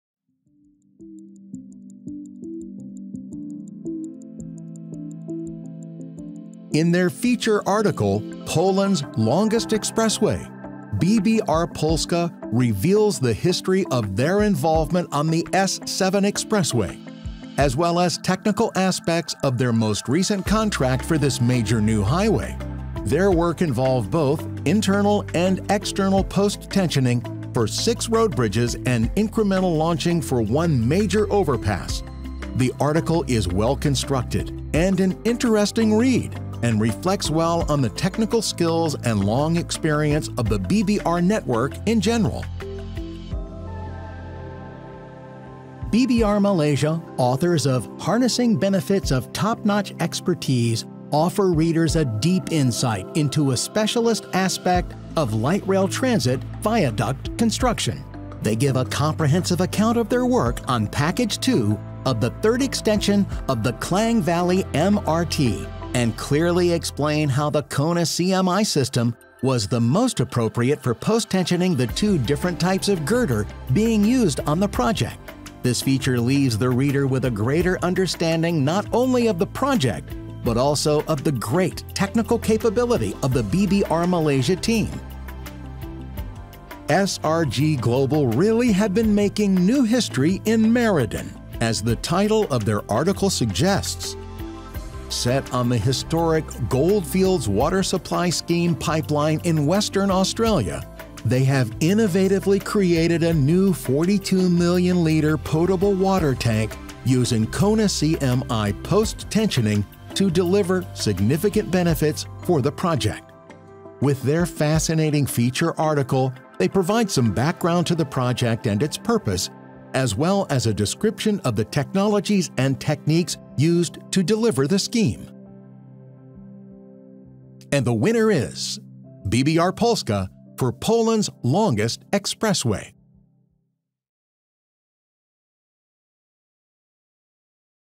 BBR CONNÆCT Best Article Award 2022
Please join us in congratulating BBR Polska on winning the BBR Network CONNÆCT Best Article Award for their feature article ‘Poland’s longest expressway’.
Their work on the S7 Expressway involved both internal and external post-tensioning for six road bridges and incremental launching for one major overpass. The story is set in the context of their long history of involvement on various sections of this new highway.
The two other shortlisted articles were BBR Malaysia’s ‘Harnessing benefits of top-notch expertise’ about their specialist experience of light rail transit viaduct construction as used on Package 2 of the Klang Valley MRT and ‘Making new history in Merredin’ by SRG Global which outlined how they used post-tensioning to deliver significant benefits for the construction of a new 42 million liter potable water tank on the Goldfields Water Supply Scheme pipeline in Western Australia.
All three articles reflect the deep technical expertise and dedication to customer service within the BBR Network - you can watch the video here - or read the full stories in CONNÆCT 2022.